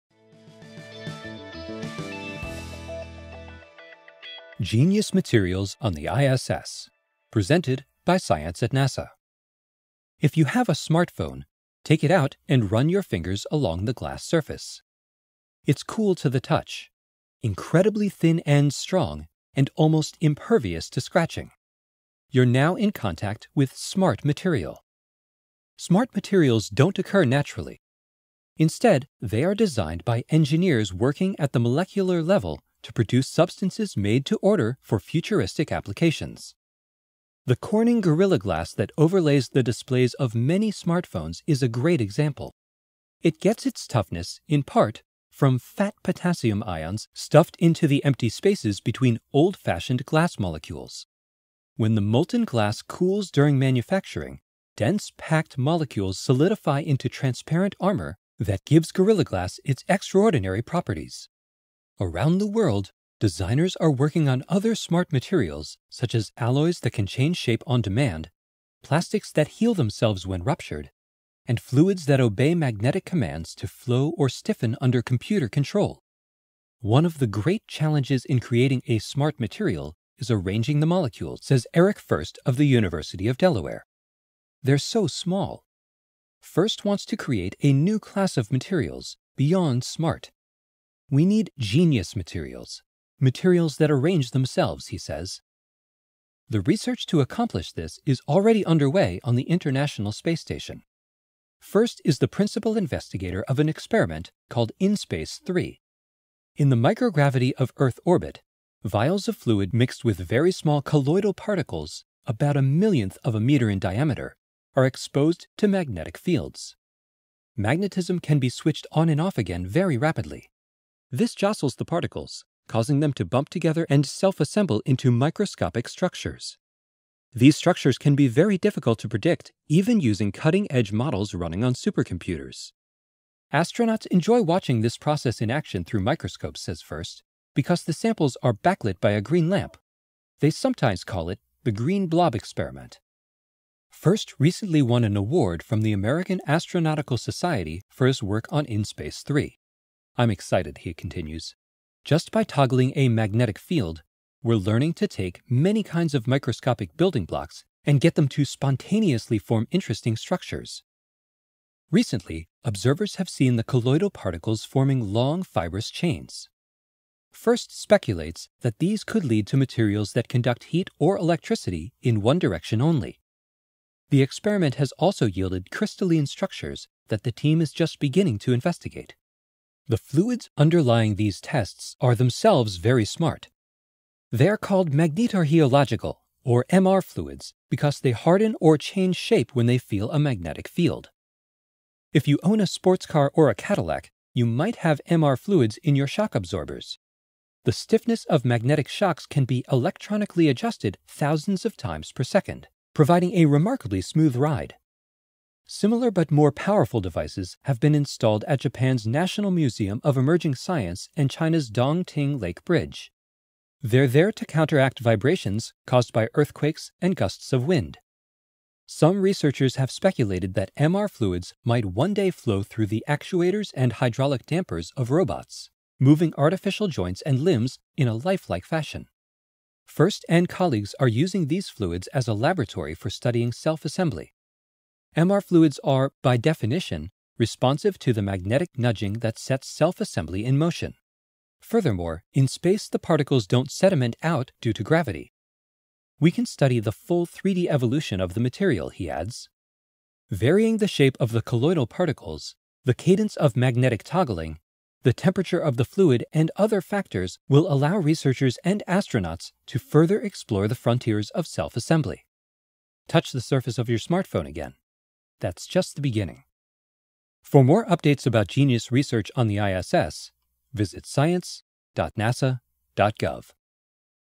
Working With "Genius Materials" on the Space Station | NASA ISS Science HD
Using magnetism, molecules can assemble themselves into "genius materials" - forming structures with amazing properties. Work on these magnetic fluids is now taking place on the space station. Please rate and comment, thanks!
Credit: NASA